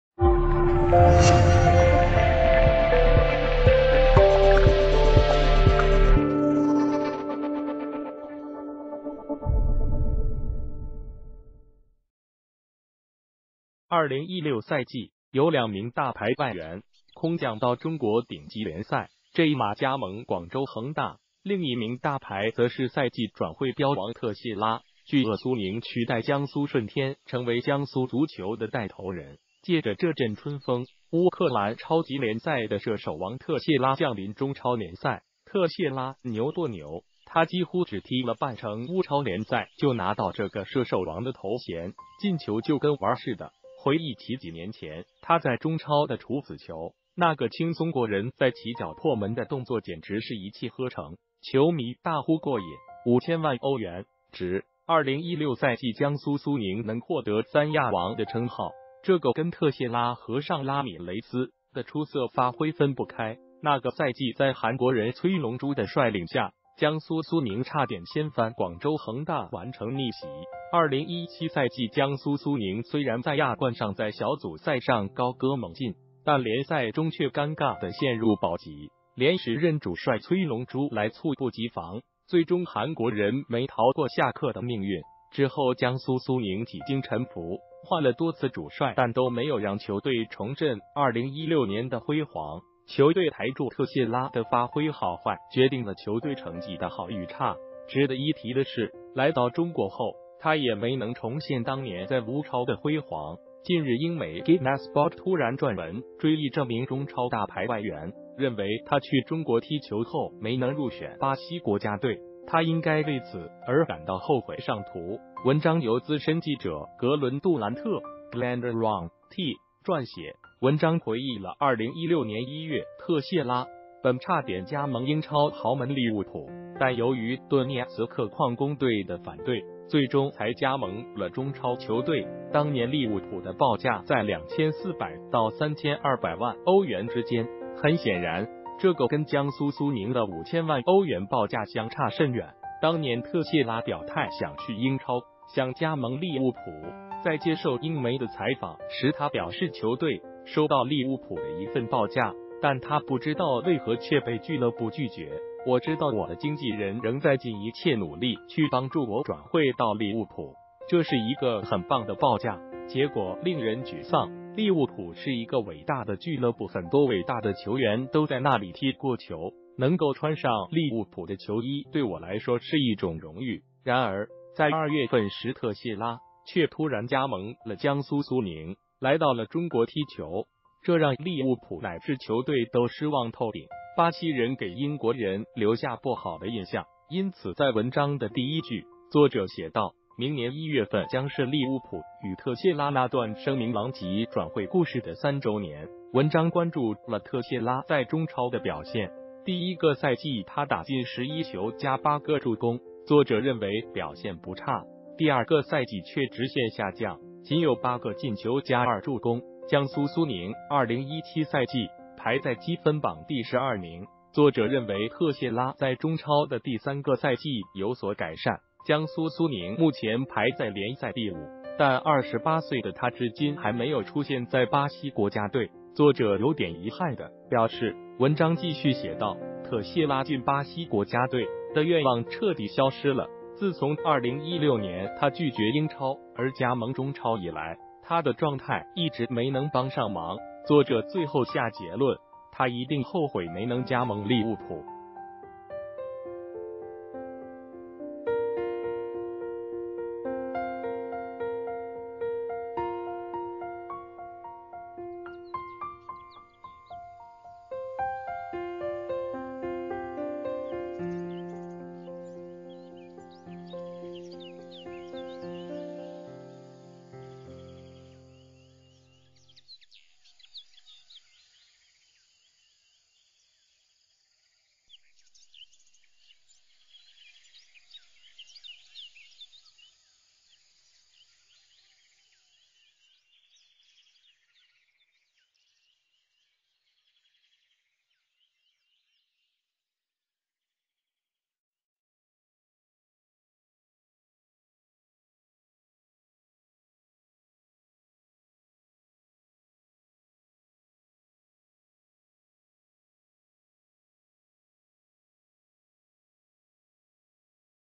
酸？外媒批中超大牌外援：无缘巴西国家队，你一定后悔没来英超
谢谢！2016赛季，有两名大牌外援空降到中国顶级联赛。J马加盟广州恒大，另一名大牌则是赛季转会标王特谢拉。巨鳄苏宁取代江苏舜天成为江苏足球的带头人，借着这阵春风，乌克兰超级联赛的射手王特谢拉降临中超联赛。特谢拉牛不牛？他几乎只踢了半程乌超联赛就拿到这个射手王的头衔，进球就跟玩儿似的。回忆起几年前他在中超的处子球，那个轻松过人再起脚破门的动作简直是一气呵成，球迷大呼过瘾，5000万欧元？值！2016赛季江苏苏宁能获得“三亚王”的称号，这个跟特谢拉和上拉米雷斯的出色发挥分不开。那个赛季在韩国人崔龙洙的率领下，江苏苏宁差点掀翻广州恒大完成逆袭。2017赛季江苏苏宁虽然在亚冠上在小组赛上高歌猛进，但联赛中却尴尬地陷入保级。连时任主帅崔龙洙来猝不及防，最终韩国人没逃过下课的命运。之后江苏苏宁几经沉浮，换了多次主帅但都没有让球队重振2016年的辉煌。球队台柱特谢拉的发挥好坏决定了球队成绩的好与差。值得一提的是，来到中国后，他也没能重现当年在乌超的辉煌。近日英媒“givemesport”突然撰文追忆这名中超大牌外援，认为他去中国踢球后没能入选巴西国家队，他应该为此而感到后悔（上图）。文章由资深记者格伦杜兰特（GlenDurrant）撰写。文章回忆了2016年1月特谢拉本差点加盟英超豪门利物浦，但由于顿涅茨克矿工队的反对，最终才加盟了中超球队。当年利物浦的报价在2400~3200万欧元之间，很显然，这个跟江苏苏宁的5000万欧元报价相差甚远。当年特谢拉表态想去英超，想加盟利物浦。在接受英媒的采访时他表示球队收到利物浦的一份报价，但他不知道为何却被俱乐部拒绝。“我知道我的经纪人仍在尽一切努力去帮助我转会到利物浦。这是一个很棒的报价，结果令人沮丧。利物浦是一个伟大的俱乐部-很多伟大的球员都在那里踢过球。能够穿上利物浦的球衣对我来说是一种荣誉。”然而，在2月份时特谢拉却突然加盟了江苏苏宁，来到了中国踢球。这让利物浦乃至球队都失望透顶，巴西人给英国人留下不好的印象。因此在文章的第一句，作者写道：“明年1月份将是利物浦与特谢拉那段声名狼藉转会故事的三周年。”文章关注了特谢拉在中超的表现，第一个赛季他打进11球+8个助攻，作者认为表现不差。第二个赛季却直线下降，仅有8个进球+2助攻，江苏苏宁2017赛季排在积分榜第12名。作者认为特谢拉在中超的第三个赛季有所改善，江苏苏宁目前排在联赛第五，但“28岁的他至今还没有出现在巴西国家队。”作者有点遗憾地表示。文章继续写道：“特谢拉进巴西国家队的愿望彻底消失了，自从2016年他拒绝英超而加盟中超以来，他的状态一直没能帮上忙。”作者最后下结论：“他一定后悔没能加盟利物浦！”